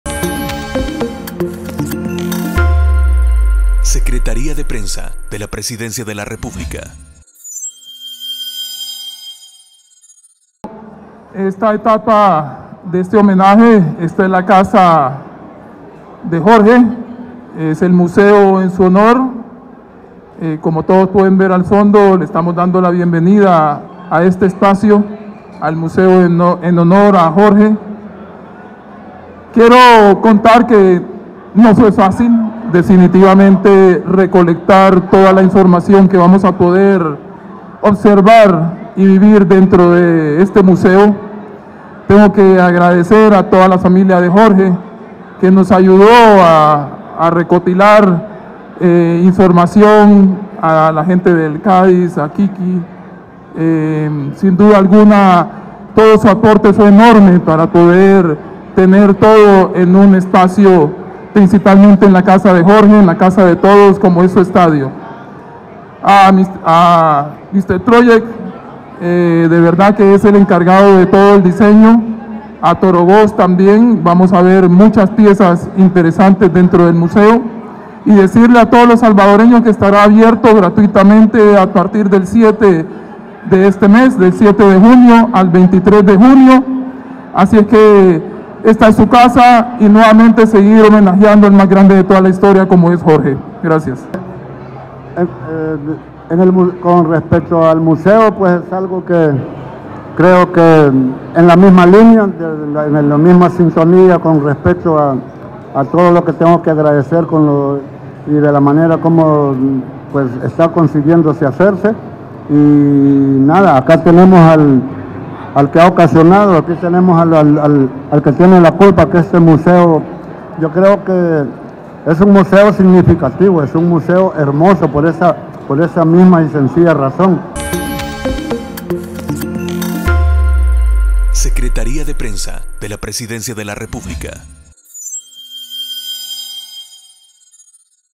Gobierno del Presidente Nayib Bukele inaugura museo en honor a Jorge “El Mágico” González
UnaNocheMágica | El presidente ad honorem del INDES, Yamil Bukele, expresa que el museo en honor a Jorge "El Mágico" González es un espacio único en su tipo en la región, el cual recorre la trayectoria del futbolista y estará abierto del 7 de junio al 23 de junio de manera gratuita.